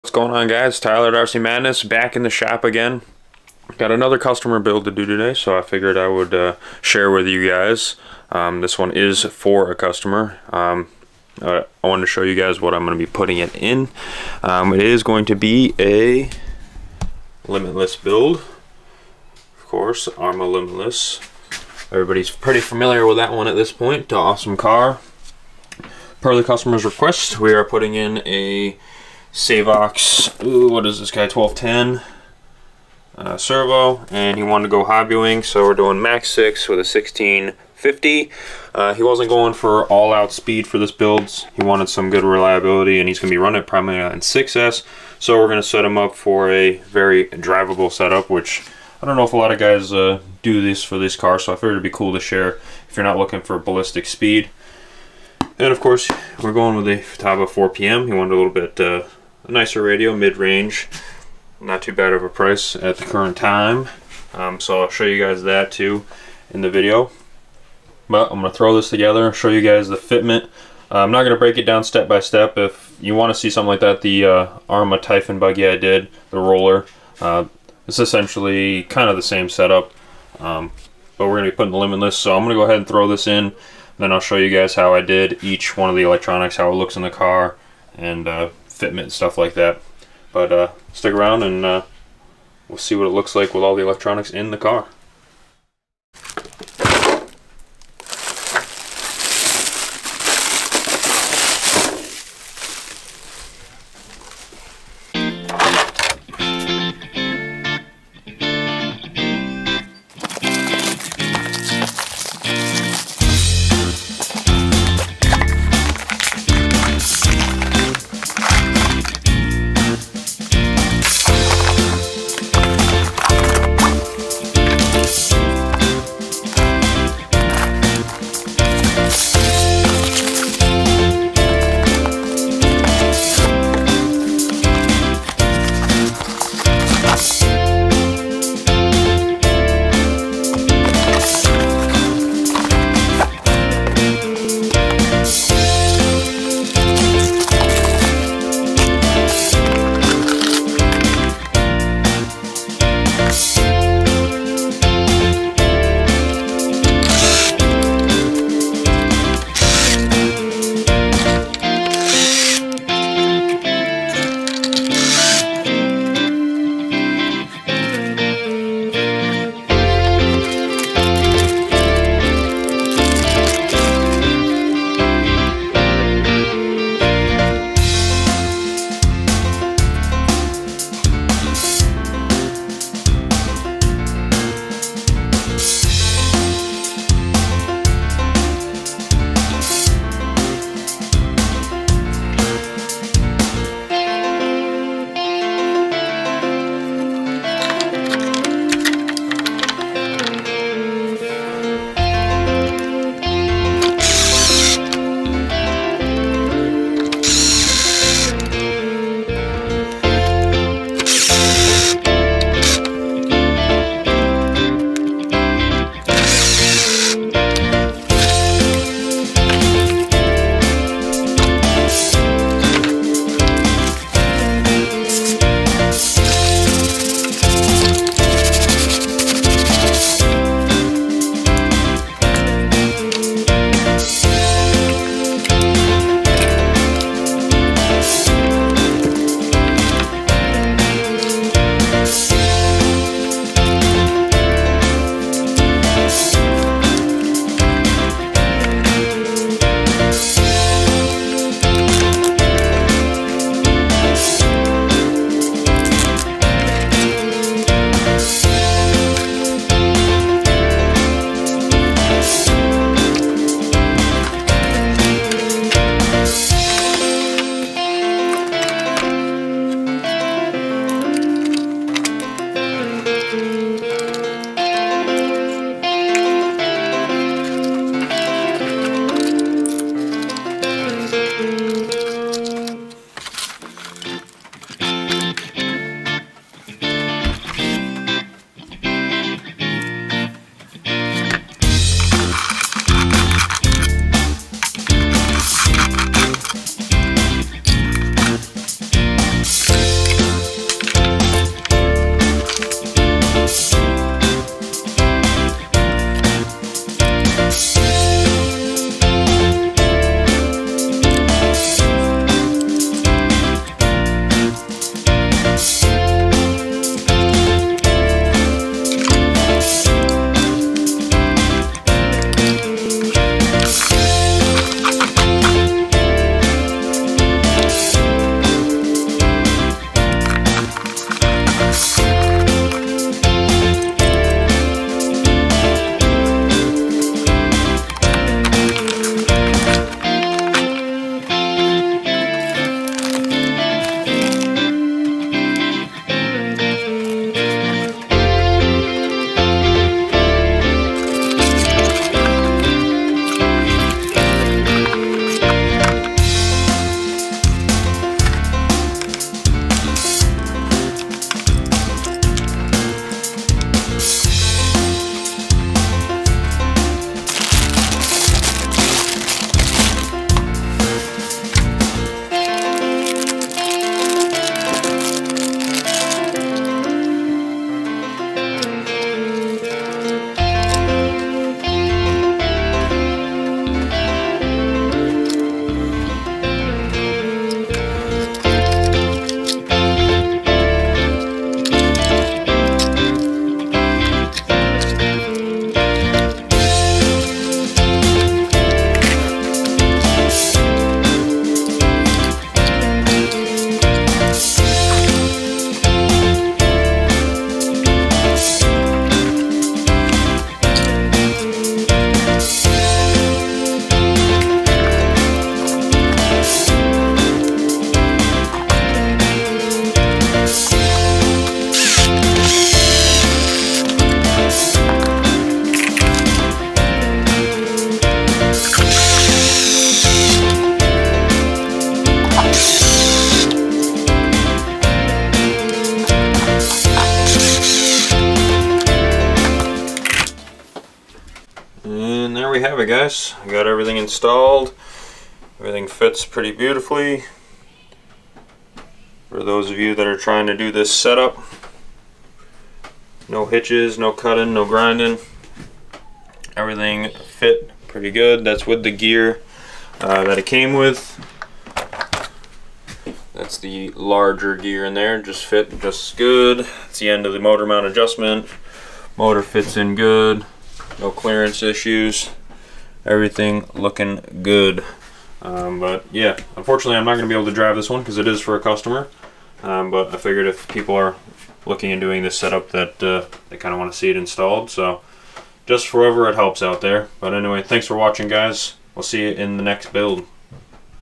Arrma Limitless Hobbywing Build Speed and Bashing Setup
A customer wanted me to build a limitless that was fast but could also be driven for more then one run.  I used a Hobbywing Max 6 and 1650kv brushless motor combo.  The servo is a savox 1210 sg water proof high torque. Paired with a Futaba 4pm radio and receiver.